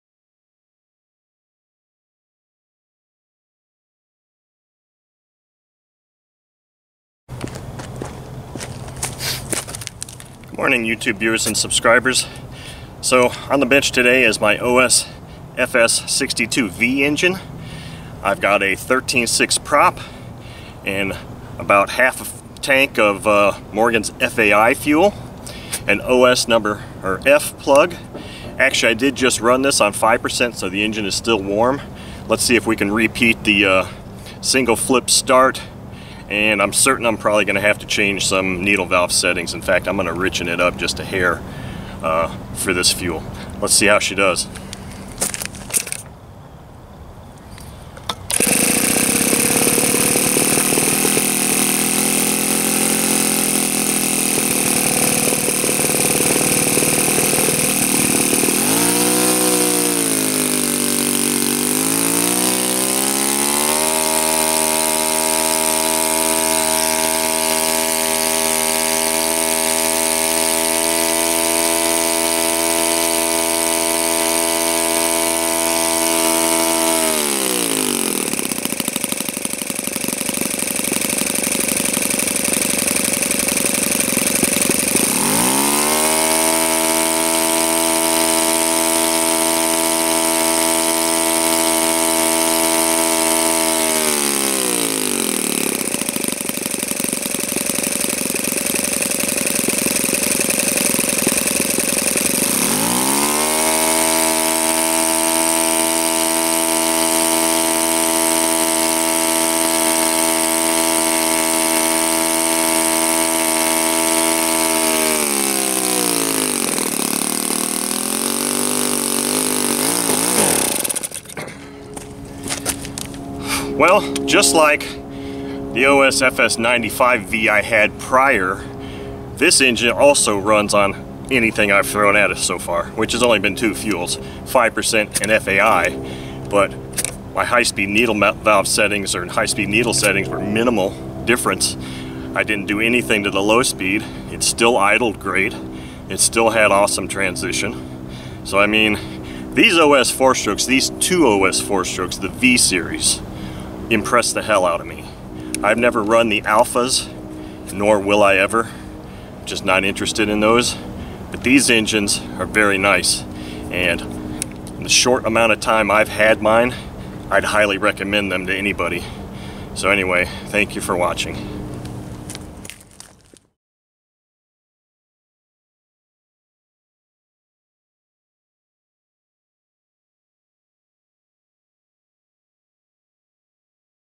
OS FS-62V FAI Fuel
Another run of this awesome engine.  This time on Omega FAI fuel, APC 13x6, and OS F plug.